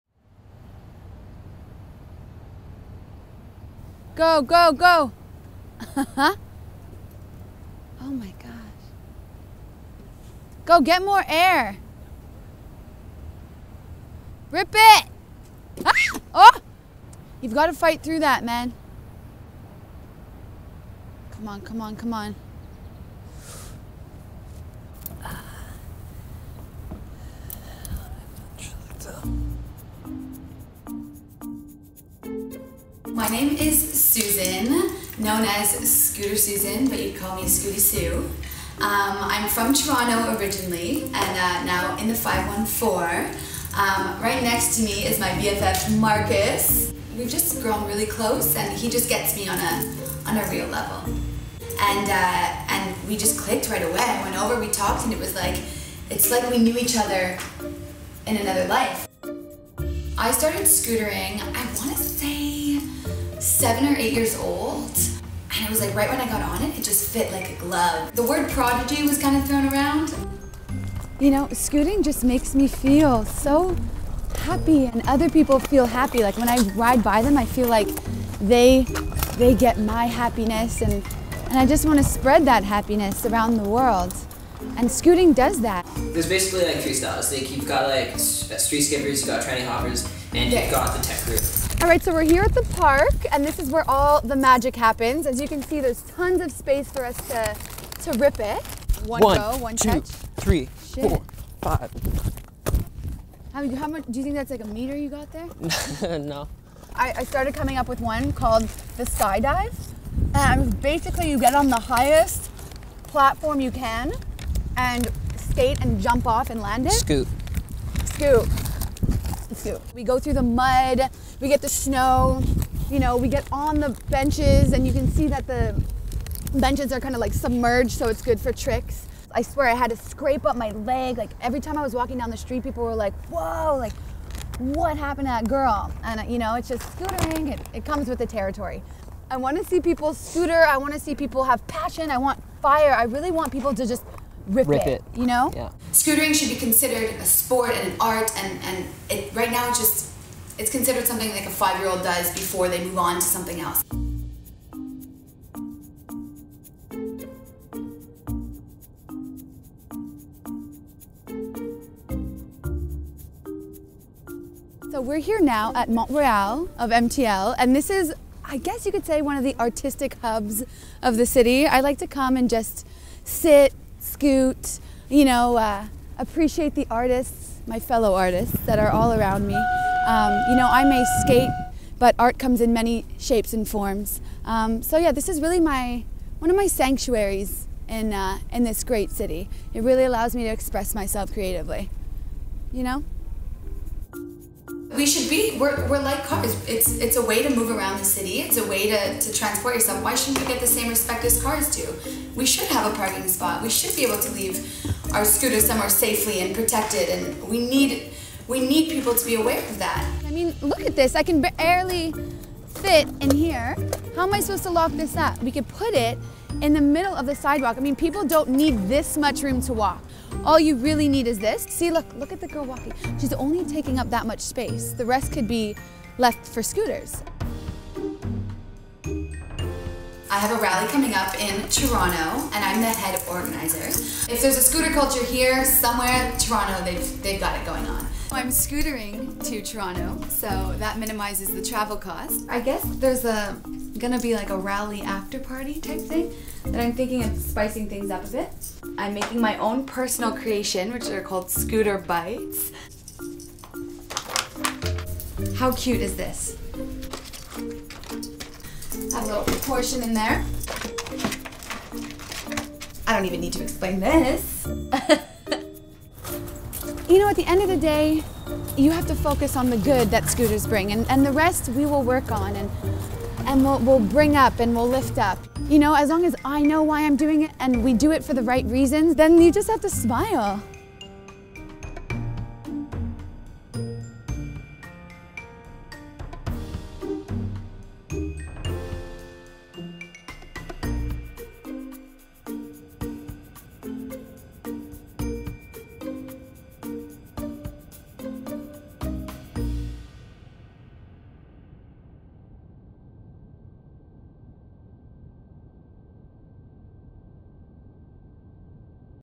SAVE SCOOTER CULTURE: Rip it
Scooter Susan, self-appointed Montreal scooter culture/rights activist, fights to protect scooter culture worldwide with the help of fellow scooterer Marcus. Their goal: to get everyone everywhere to RIP IT.